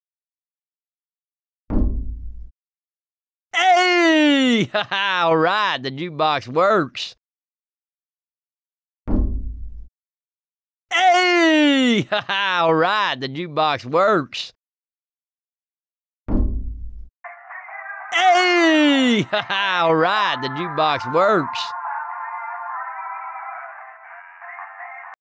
Short Test Animation (Ellis "Happy Days" JukeBox)
Through out the progress of this short test animation I was re-learning SFM.